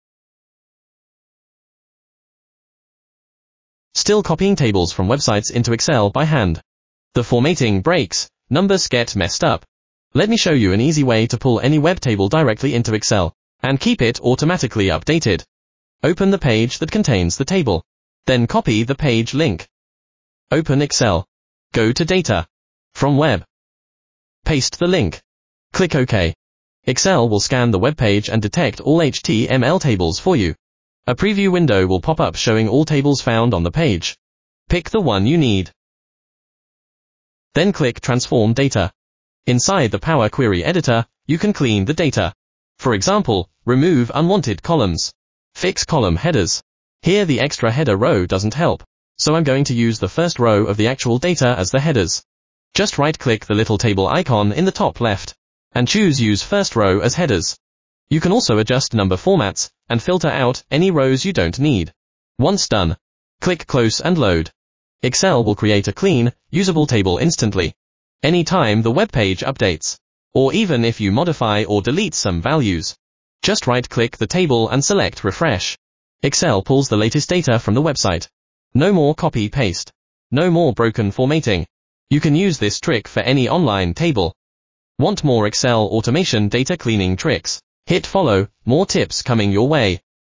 Import Data from Web into Excel
Stop copying and pasting web tables manually!
This tutorial shows how to use Excel’s built-in “Get Data from Web” feature to import HTML tables directly from websites.
The best part? The imported data can automatically refresh whenever the source page updates.
Perfect for financial data, sports stats, or any live web tables you want in Excel!